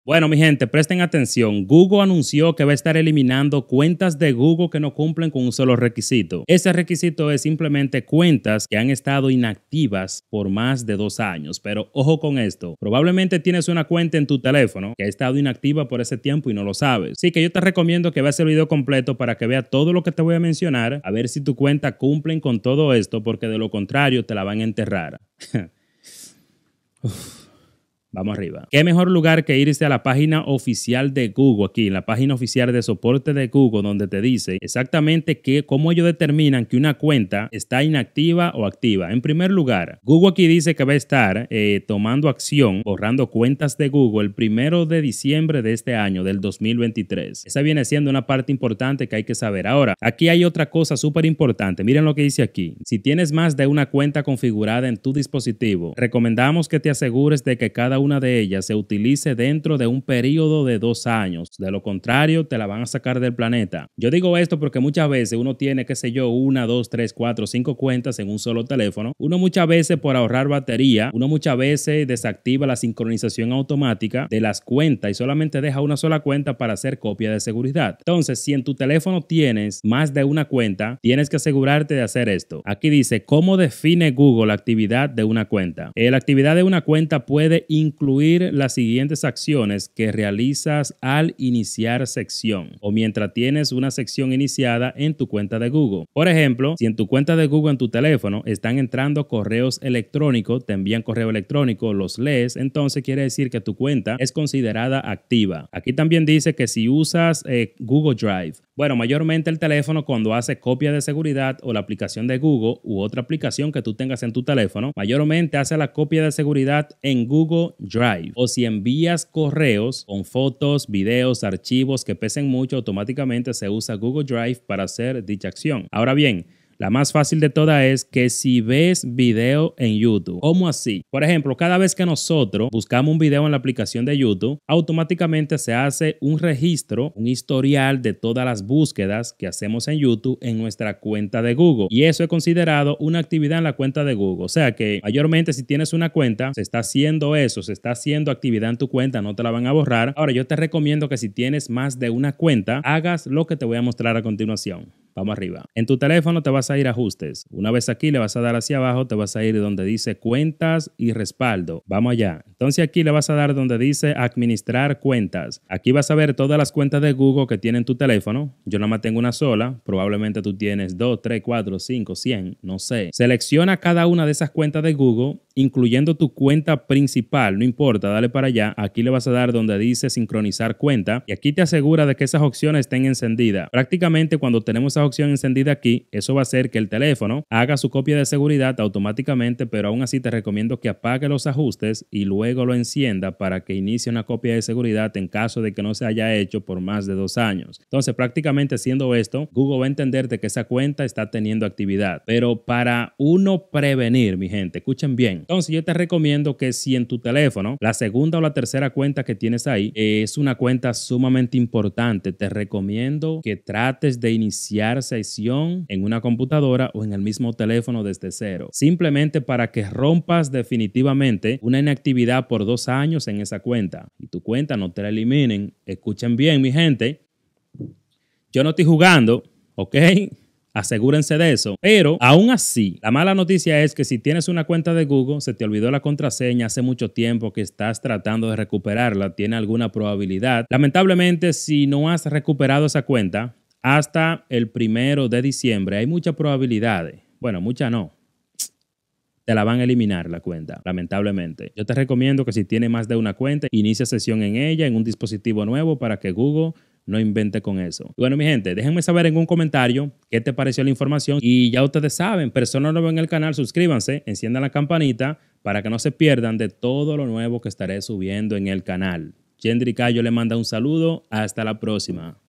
Perderás Tu Cuenta de Google si no Cumple con este requisito ( Revisa HOY )
Estas son las cuenta que google va a eliminar si no cumplen con este requisito!